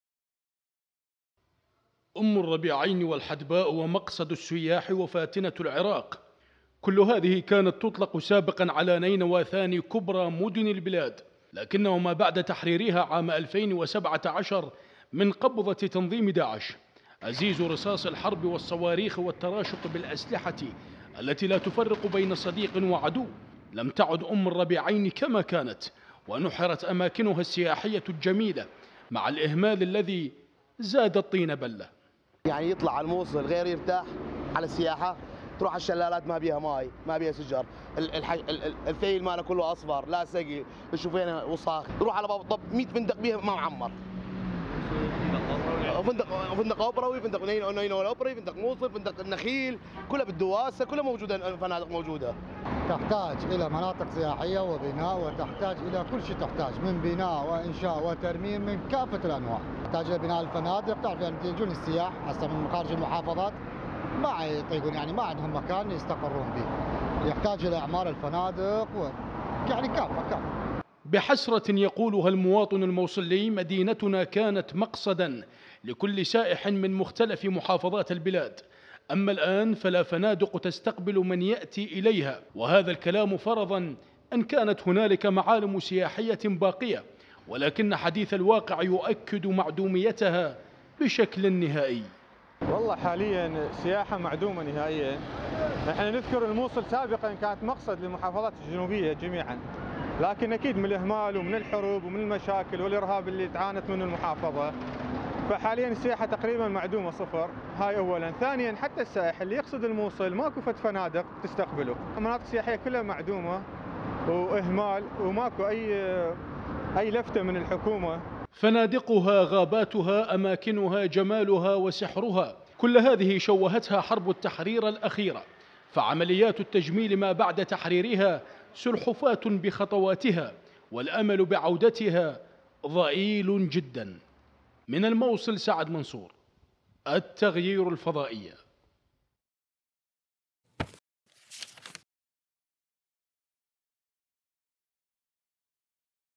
فاتنة العراق بحاجة إلى عمليات تجميل مستعجلة | تقرير
فاتنة العراق بحاجة إلى عمليات تجميل مستعجلة
رغم انها كانت واحدة من أجمل مدن العراق. إلا أن الحرب والإهمال نالا منها وجعلاها خاوية على عروشها، أم الربيعين الموصل الحدباء تشكو انعدام الأماكن السياحية رغم مرور ثلاث سنوات على تحريرها.